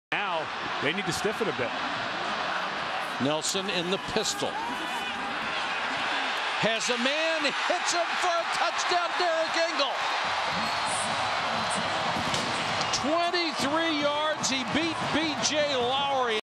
Derrick Engel 23-Yard Touchdown
Minnesota's Philip Nelson completes pass to Derrick Engel for a 23-yard touchdown in the third quarter against Iowa.